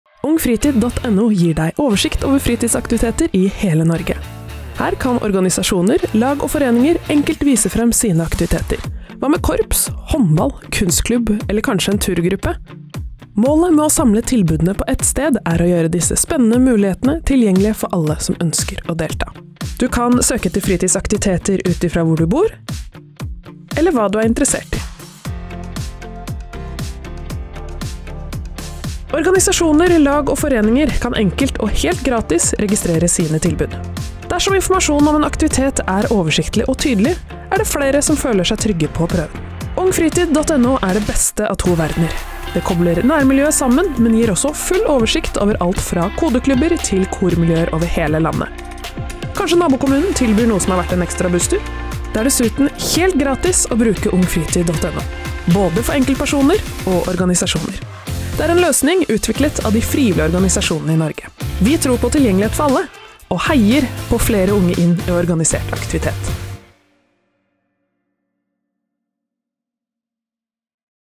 Bruk Ungfritid.no til å vise frem fritidsaktiviteter!
Ungfritid.no er en digital oppslagstavle hvor frivillige organisasjoner kan legge ut varige fritidsaktiviteter for barn og unge opptil 26 år. Siden gjør det enklere for frivillige organisasjoner å finne deltakere, og enklere for barn, ungdom, foresatte og andre å finne fritidsaktiviteter. Ungfritid.no er helt gratis å bruke.

Ungfritid.no drives av de frivillige organisasjonene selv gjennom Frivillighet Norge. Prosjektet er støtte av Sparebankstiftelsen DNB, Helsedirektoratet, Stiftelsen DAM, Gjensidigestiftelsen og Barne- ungdoms og familiedirektoratet. Ungfritid.no bygger på samme digitale plattform som Frivillig.no.

Hvem kan legge ut fritidsaktiviteter på Ungfritid.no?
- Alle organisasjoner registrert i Frivillighetsregisteret
- Frivilligsentraler
- Menigheter i Den norske kirke
- Kommunale fritidsklubber